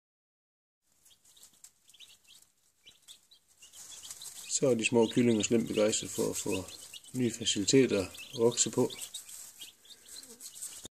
Kyllingerne vokser
Så fik kyllingerne lidt ekstra plads med udvidelses mulighed.